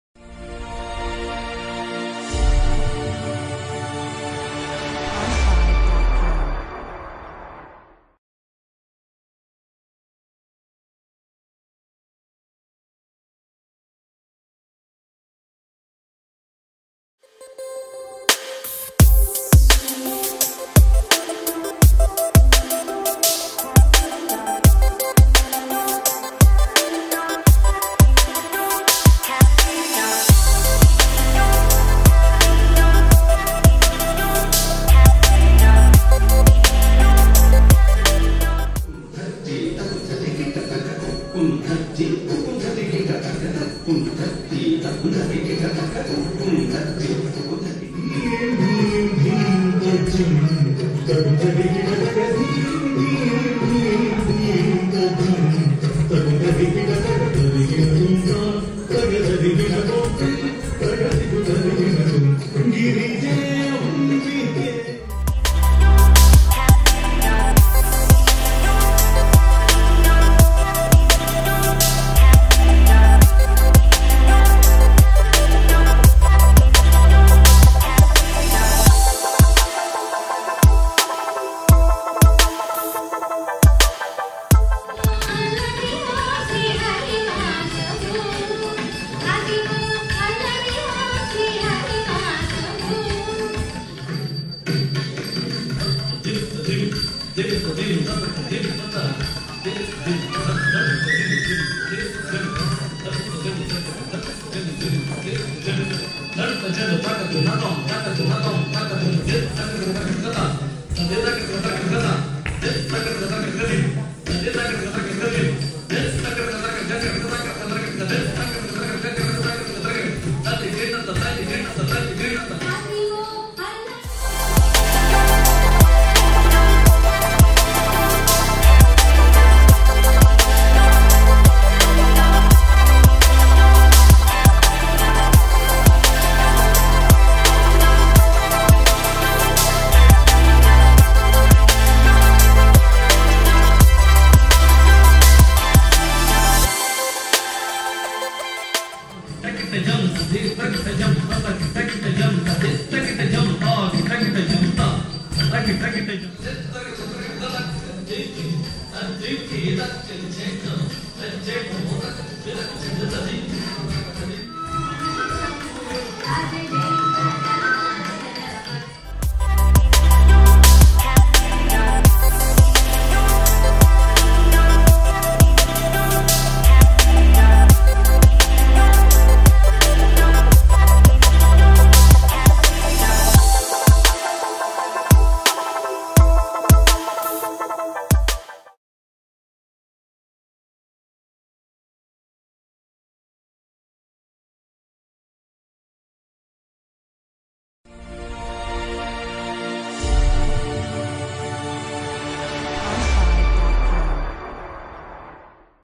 Dallas - NATS Balalu Sambaralu Highlights held on October 20th 2018.
NATS (North America Telugu Society) conducts Balalu Sambaralu 2018 at Malankara Church Auditorium on October 20th 2018.

Check our some visuals from the event.

Please visit our other pages.